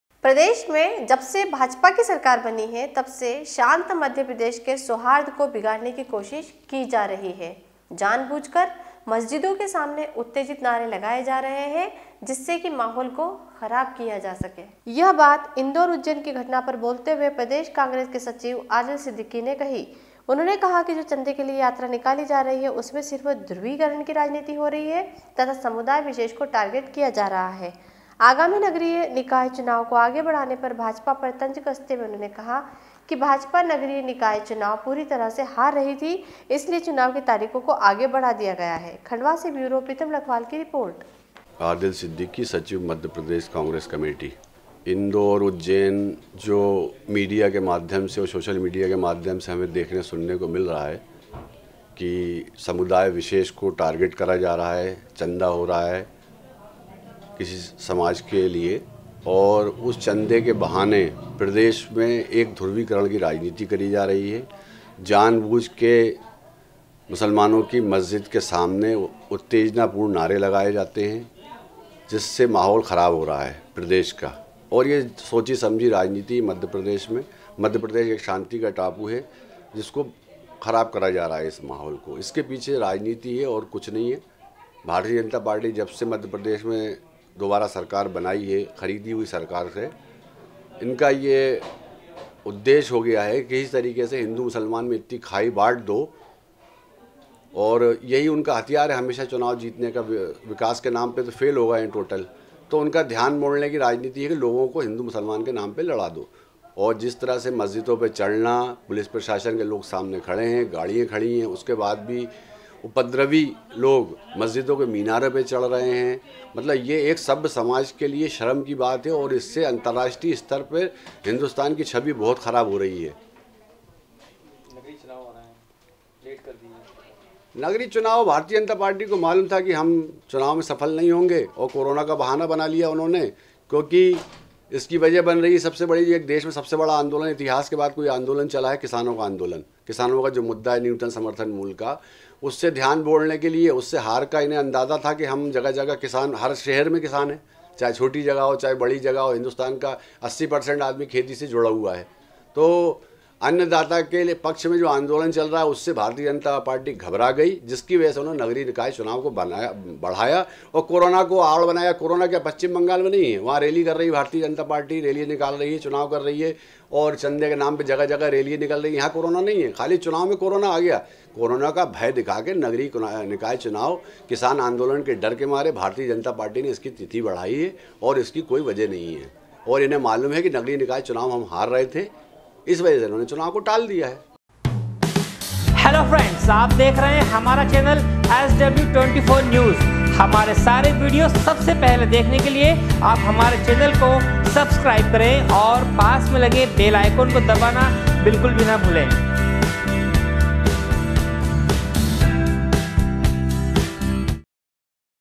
Khandwa- भाजपा की सरकार बनी है तब से शांत मध्यप्रदेश के सौहार्द को बिगाड़ने की कोशिश की जा रही है
Khandwa- SW 24 News Madhyapradesh
प्रदेश में जबसे भाजपा की सरकार बनी है तब से शांत मध्यप्रदेश के सौहार्द को बिगाड़ने की कोशिश की जा रही है। जानबूझकर मस्जिदों के सामने उत्तेजित नारे लगाए जा रहे है। जिससे कि माहौल को खराब किया जा सके। खंडवा से ब्यूरो प्रीतम लखवाल की रिपोर्ट